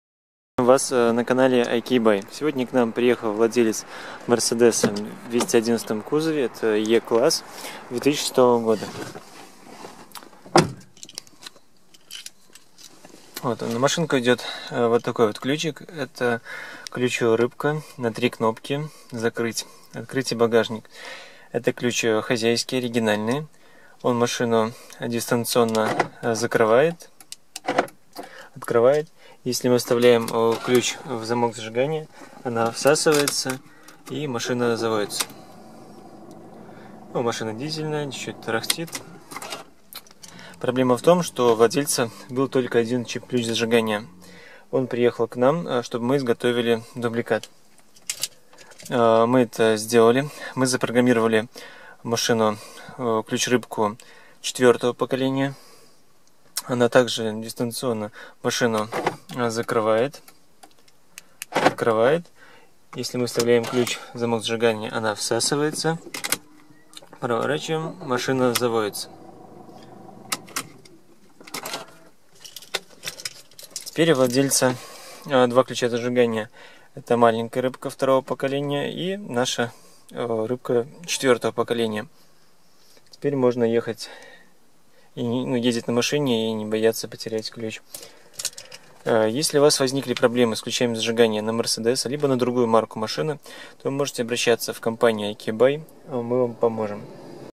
Mercedes Benz E Class w211 изготовление дубликата чип ключа зажигания рыбки в Минске Key Programming
К нам приехал владелец Мерседес w211 Е Класса 2006 года в Минск. У него на руках было один автоключ зажигания. Наши специалисты сделали дубликат чип ключа зажигания и запрограммировали в автомобиль в течении часа.

How To Program A New Key For A Mercedes w211 2006
Best OBD Mercedes Key Programmer All Lost Key